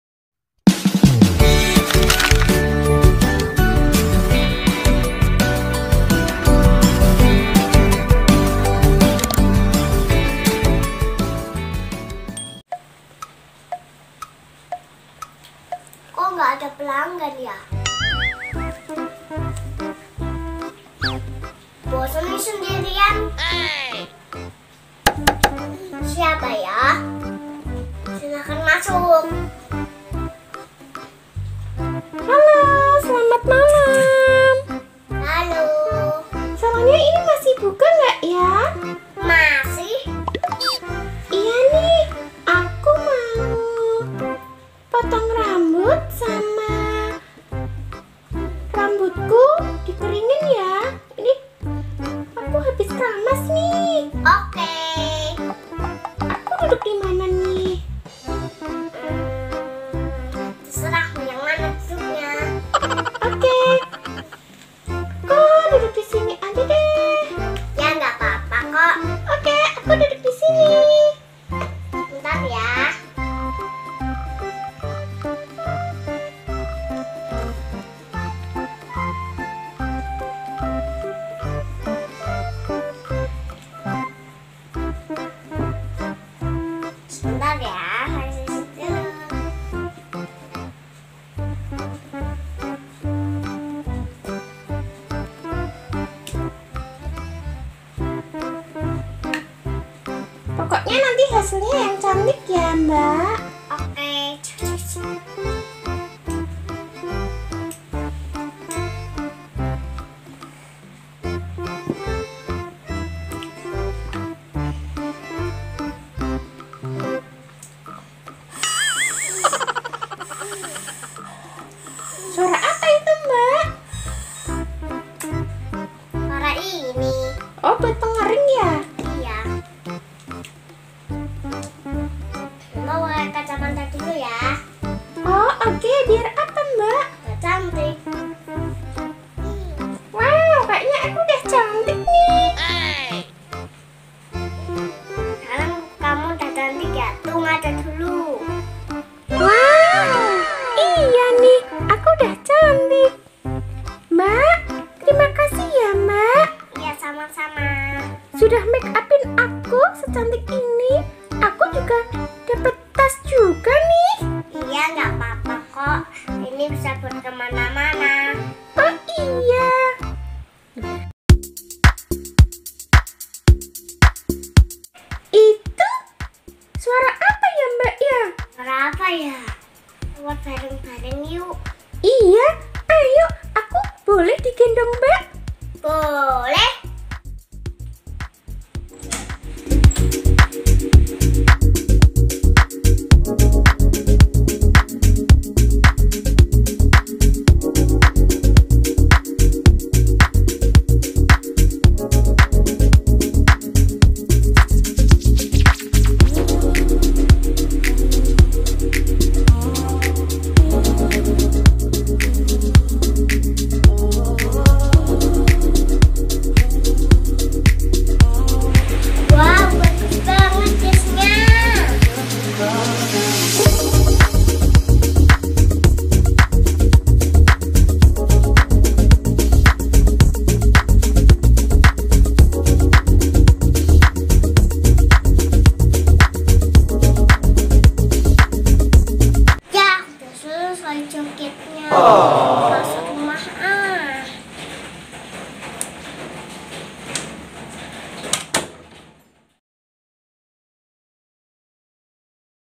MAINAN SALON SALONAN 💄 Ada FROZEN Funny Dance Di Malam Hari ❄️ Alfira Zahra Channel
Selamat Datang di Alfira Zahra Channel !
Kali ini Zahra Sebelum Tidur Mainan Salon Salonan.. tiba-tiba ada Frozen Funny Dance ..Lucu dan Seruuu banget teman-teman...